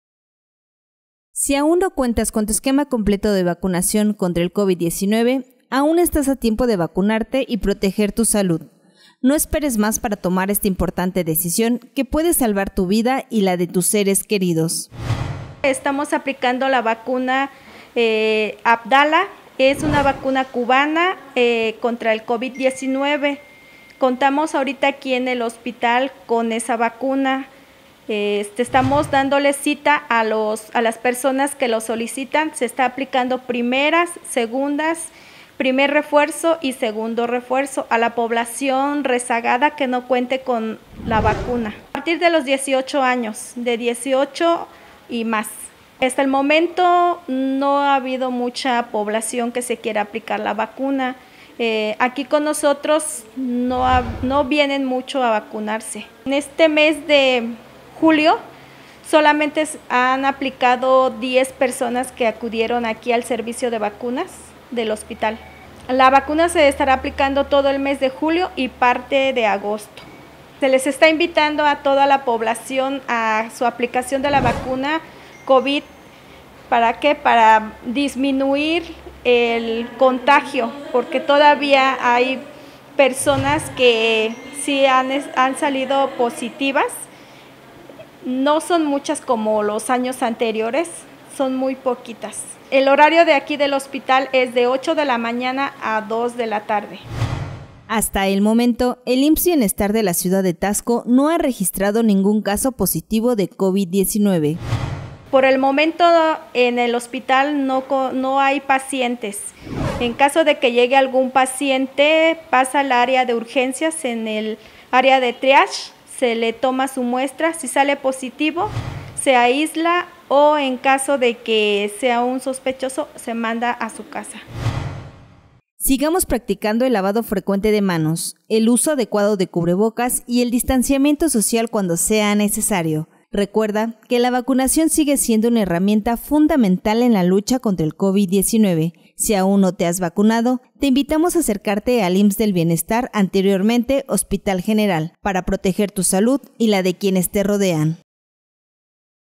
IMSS Bienestar en Taxco ofrece Vacuna Abdala
IMSS Bienestar en Taxco ofrece la Vacuna Abdala: Protégete contra el COVID-19